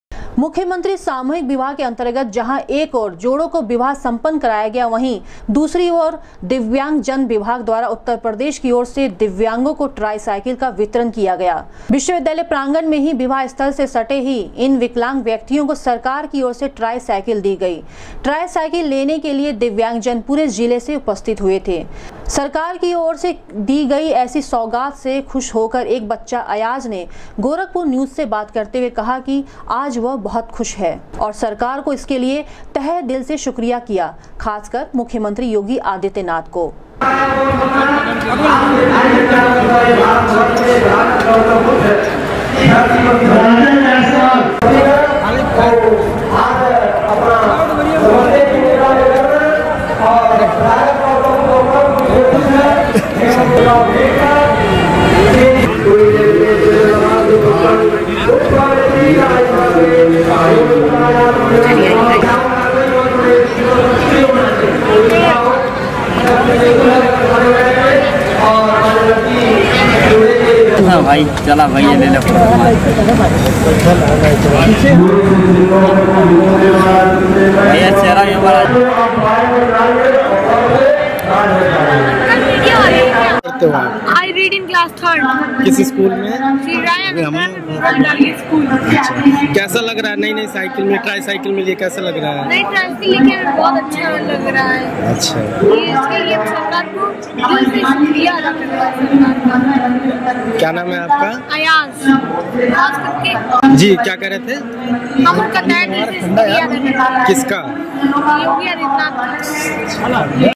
मुख्यमंत्री सामूहिक विवाह योजना के अंतर्गत की विश्व विद्यालय प्रांगण में बांटा गया दिव्यांग जनों को
मुख्यमंत्री सामूहिक विवाह योजना के अंतर्गत की विश्व विद्यालय प्रांगण में बांटा गया दिव्यांग जनों को ट्राई साइकिल, उपहार पाकर खिले चेहरे, किया तहेदिल से शुक्रिया